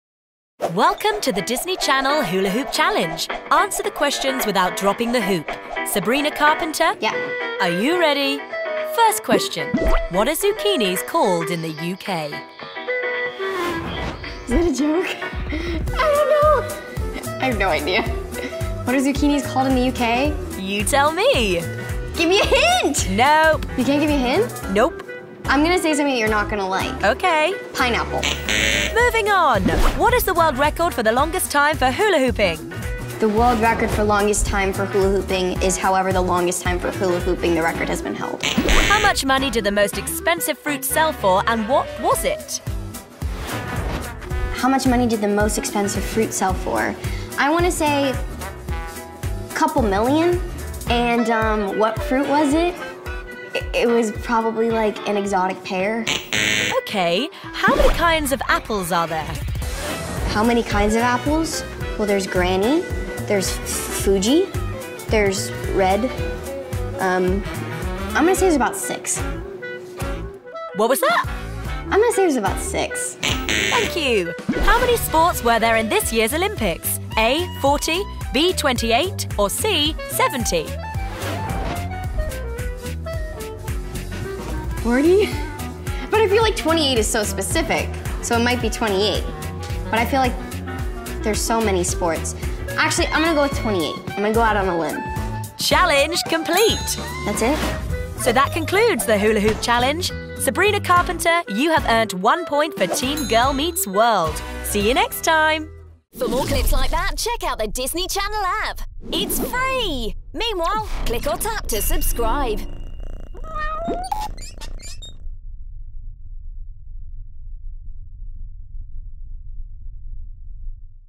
Hula Hoop Challenge | Sabrina Carpenter | Official Disney Channel UK
It's Sabrina Carpenter's, from Girl Meets World, turn to take on Disney Channel's hula hoop challenge!

Android user?
iOS user?